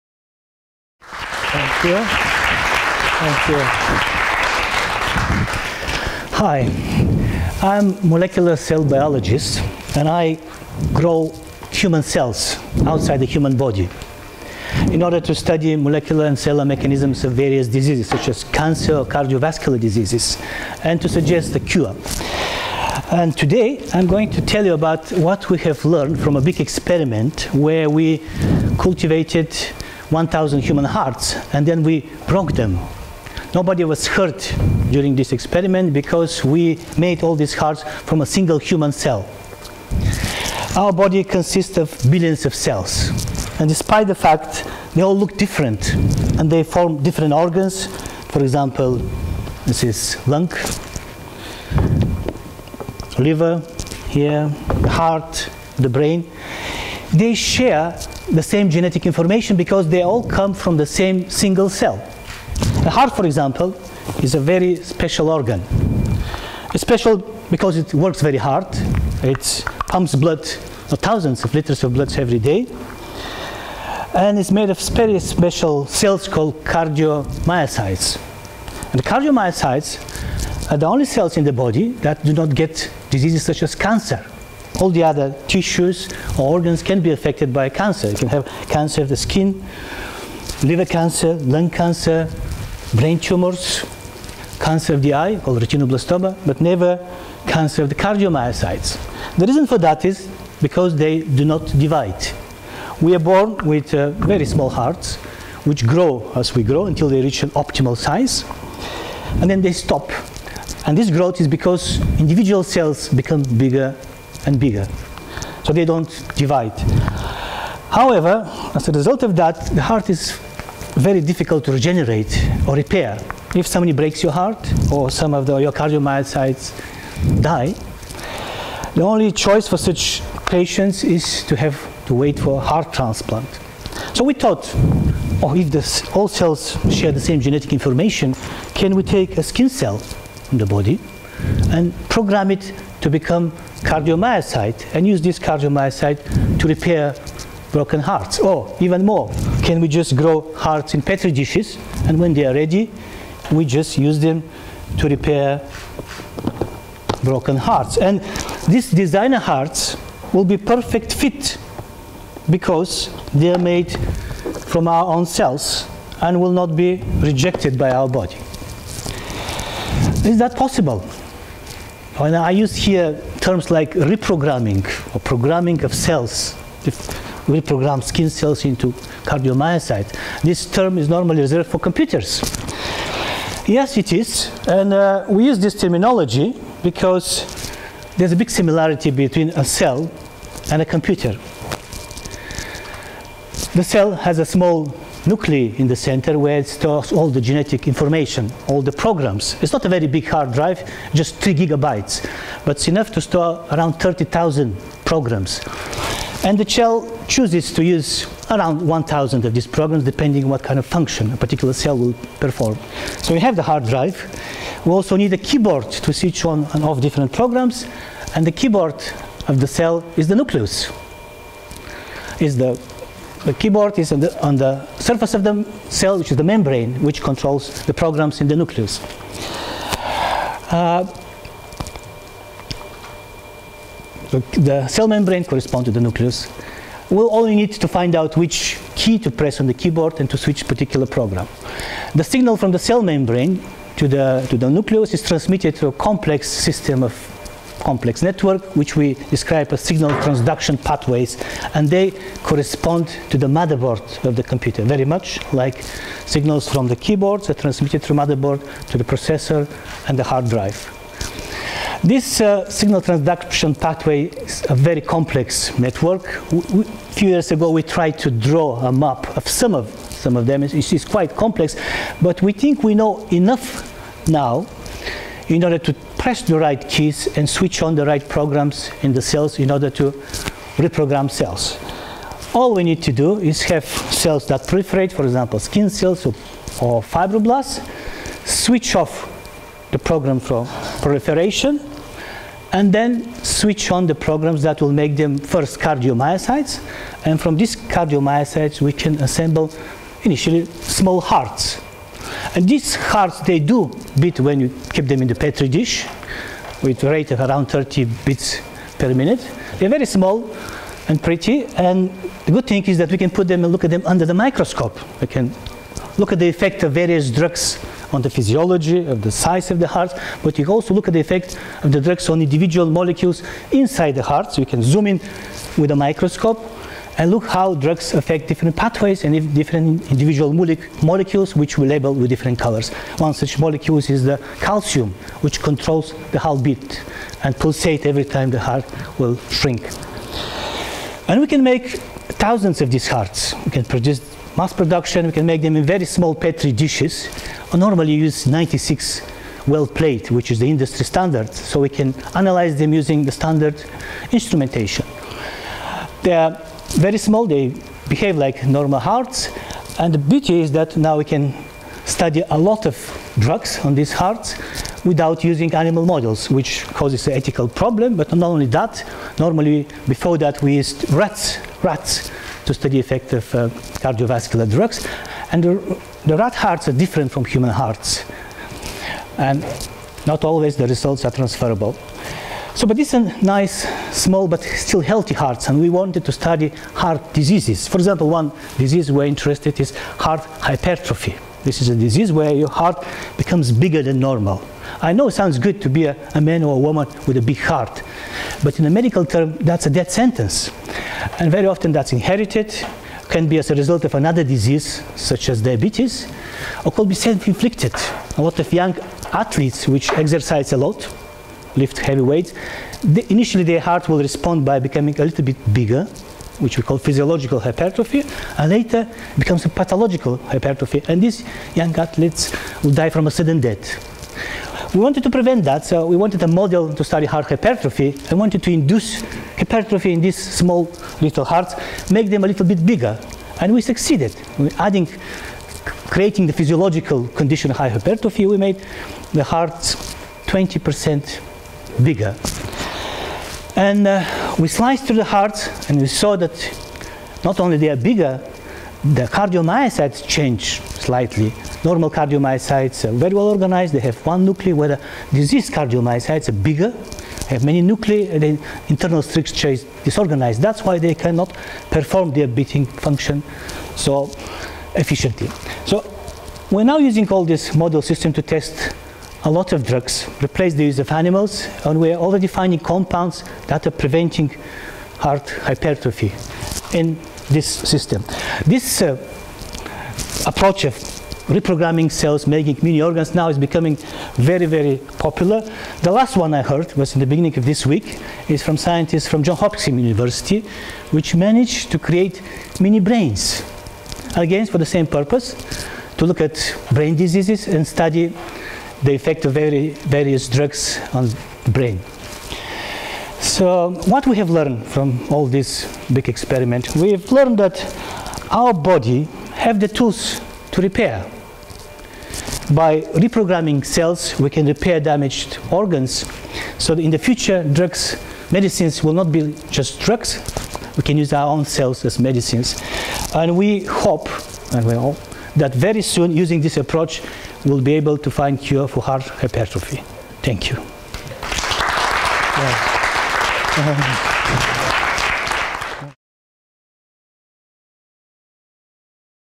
How to break a thousand hearts | Nikolai Zhelev | TEDxAbertayUniversity
In his talk Professor Zhelev explains what we can learn from making and breaking a thousand human hearts. Abertay University researchers have developed tiny human hearts, just 1 mm in diameter, which beat on their own accord. They are made from stem cells, which are reprogrammed to become cardiomyocytes (cells that make up the heart). Although healthy to begin with, Professor Zhelev and colleagues are using chemicals to simulate the physiological conditions that will induce diseases such as heart hypertrophy in them. Heart hypertrophy is the most common cause of sudden death in young athletes and there is no known cure for the disease at the moment. However, it is hoped the miniature human hearts could help change that.

Dr Nikolai Zhelev is Professor of Medical Biotechnology at Abertay University. He is also honorary professor in eight universities in UK, China and Bulgaria. Prof. Zhelev has been involved in founding four start-up biotech companies. He has published 3 books and more than 80 research papers and patents in the field of DNA damage response, cell cycle regulation and drug discovery and development in oncology and cardiology in reputed journals such as Nature Medicine. Prof Zhelev is Managing Editor of BioDiscovery – the first open access international scientific journal in the field of Life Sciences and Medicine, published in Scotland and currently the President of the European BioDiscovery Federation.